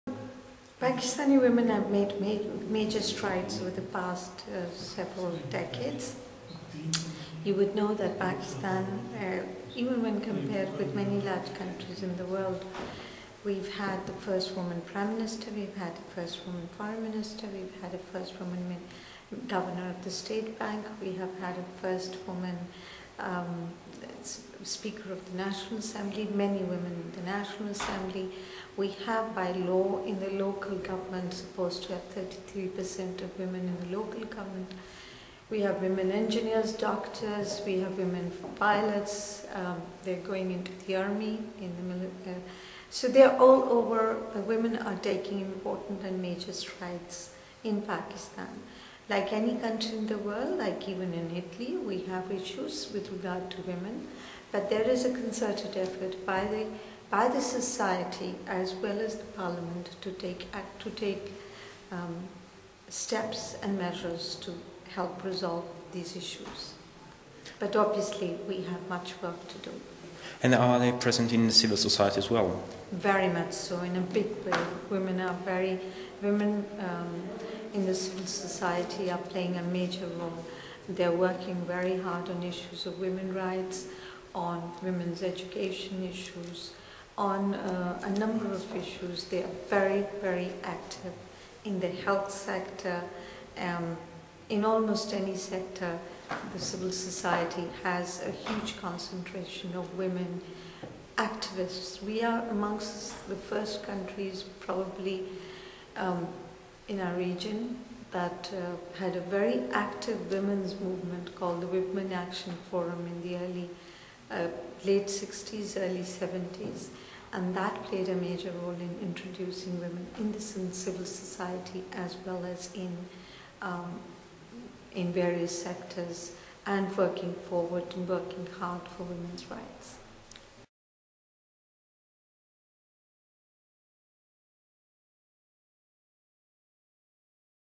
Pakistan: a model for gender equality
We talk with H.E. Tehmina Janjua, Pakistani Ambassador to Italy, about the role of women in Pakistan's society.